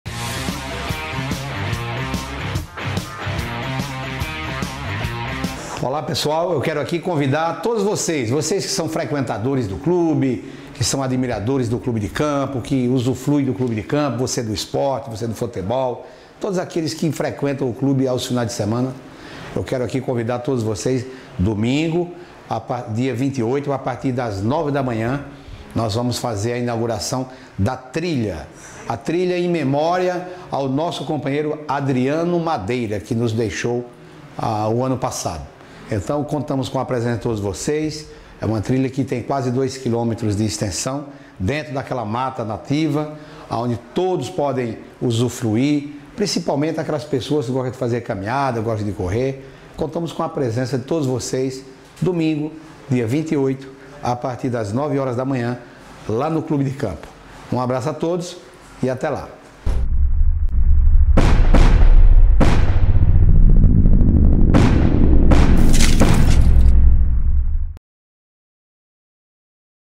CABEÇA CONVIDA PRA INAUGURAÇÃO DA TRILHA
Localizada dentro do nosso Clube de Campo (Parque Primavera), a Trilha Madeira será inaugurada domingo, dia 28, a partir das 9 horas. Será 1,5 km de contato com a natureza, respirando ar puro e apreciando a bela paisagem. Espaço tem grau de dificuldade leve. Assista!